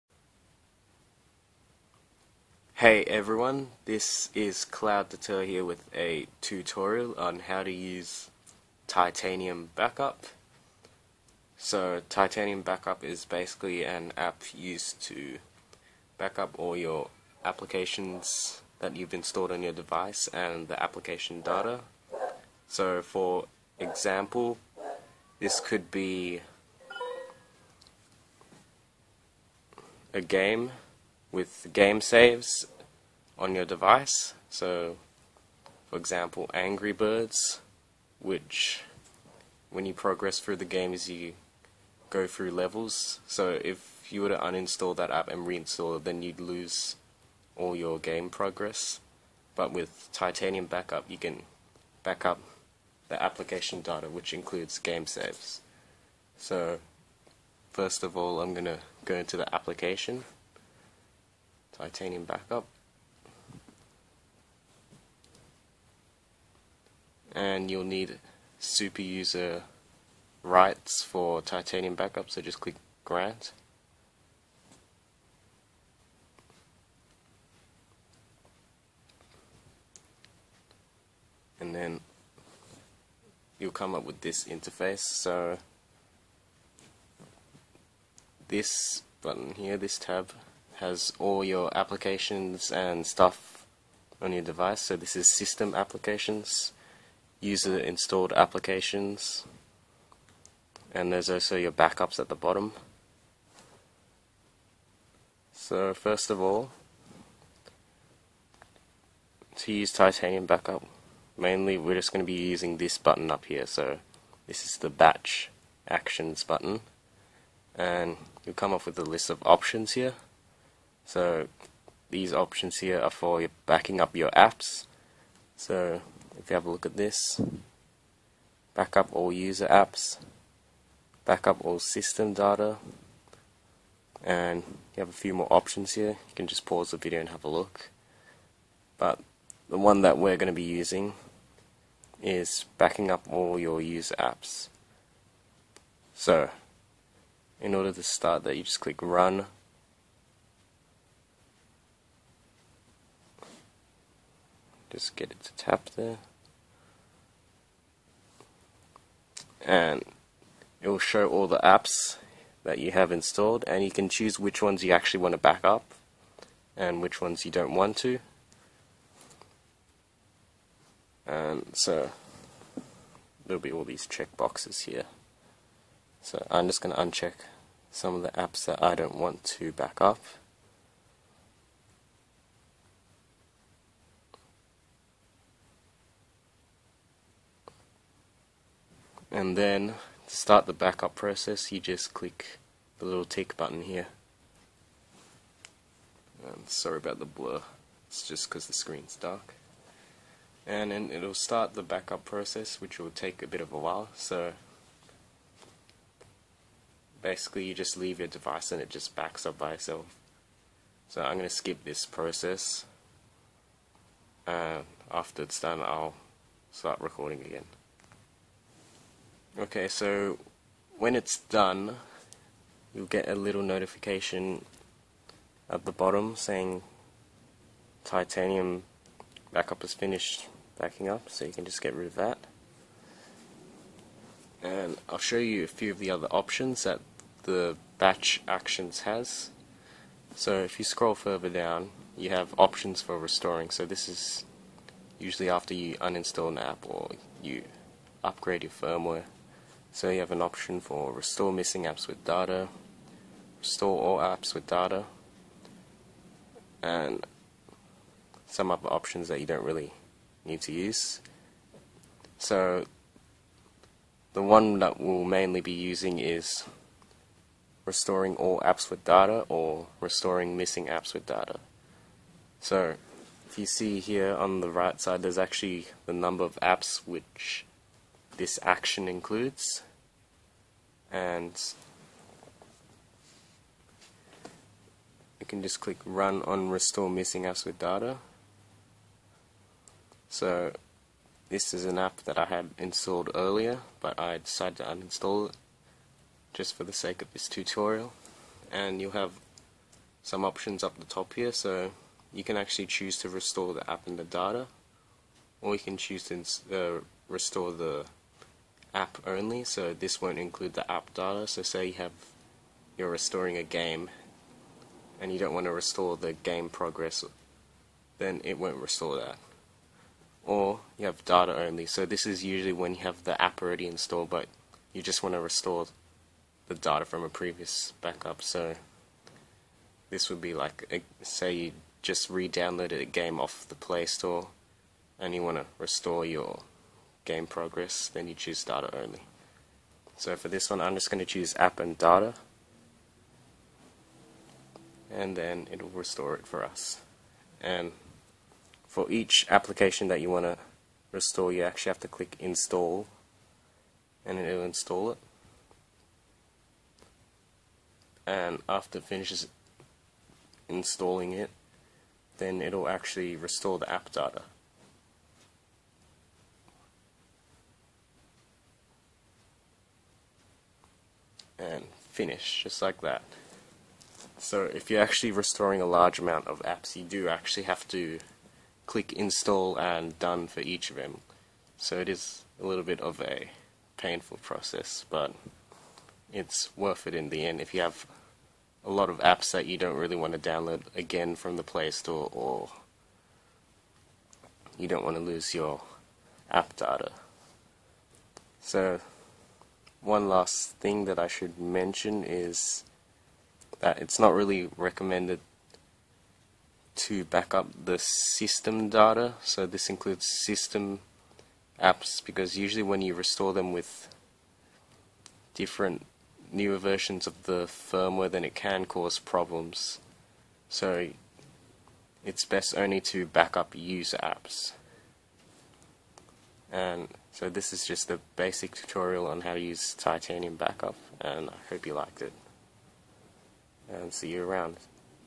How to: Use Titanium Backup on the Ainol Novo 7 Venus
A tutorial on using Titanium Backup on the Ainol Novo 7 Venus tablet. Titanium Backup is used for backing up applications installed on your device, including the app data, and for restoring backups at a later date.